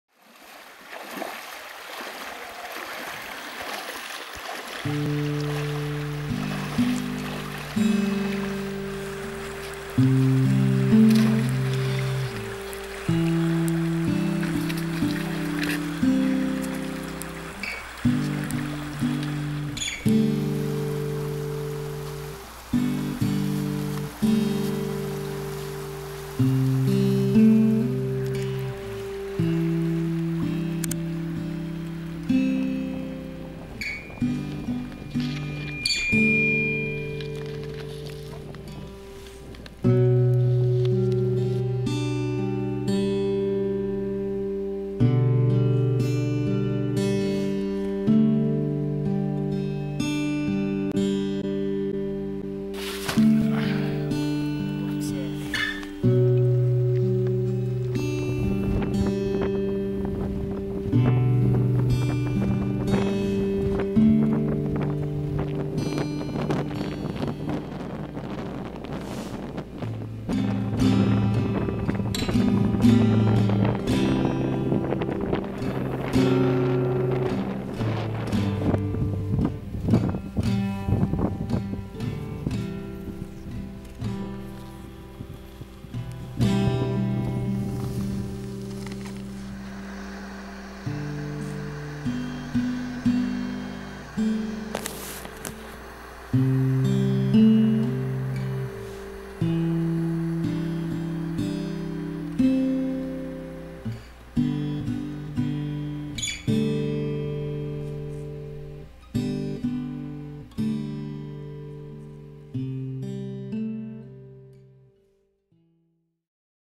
Haines Alaska, Jarvis-Tsriku Loop
Spring Break! We drove to Haines, got skunked with cold, wind, and water.